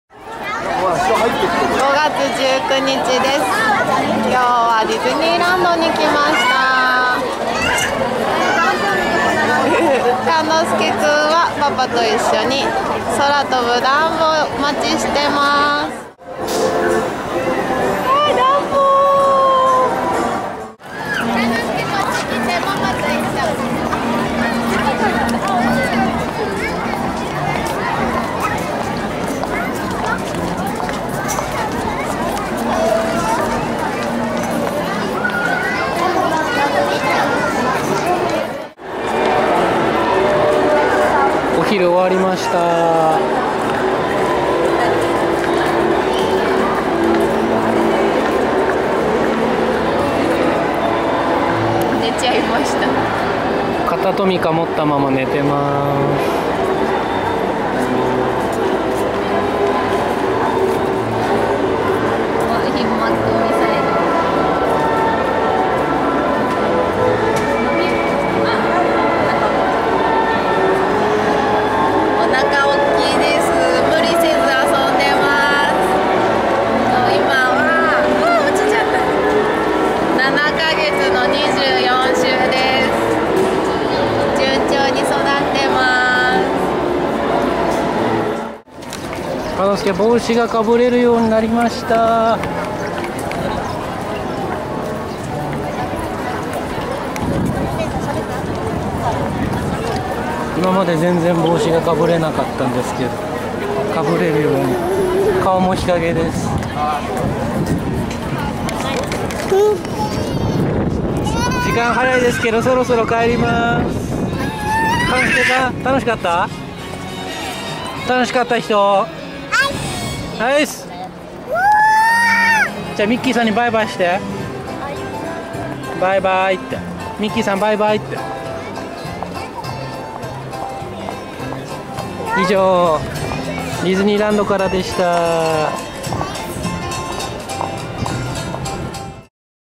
東京ディズニーランド
東京ディズニーランドに行ってきました、今日の成果はなんと言っても「帽子をかぶった」ことですね。絶対にかぶらなかった帽子...夏にむけて猛特訓が必要そうですが、かぶせた瞬間に違うことを考えさせてやれば、なんとかかぶってくれるみたいです。